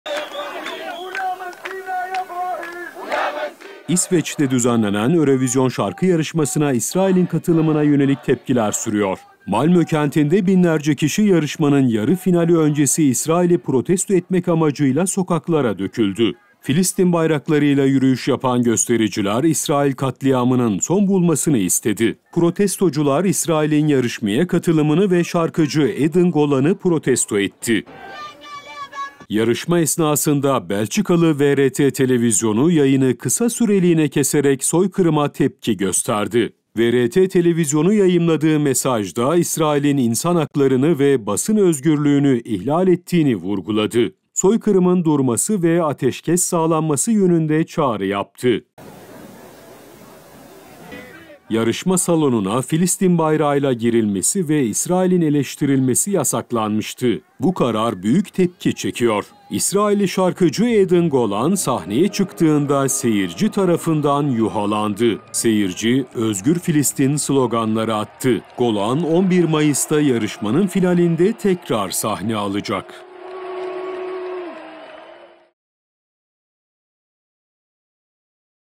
Belçika'nın VRT televizyonu, Eurovision yayını sırasında İsrail’i protesto etti
Belçika'nın VRT televizyonu, Eurovision Şarkı Yarışması yayınını bölerek İsrail'in insan hakları ve basın özgürlüğüne yönelik ihlallerini kınadığı ve ateşkes talep ettiği mesaj paylaştı.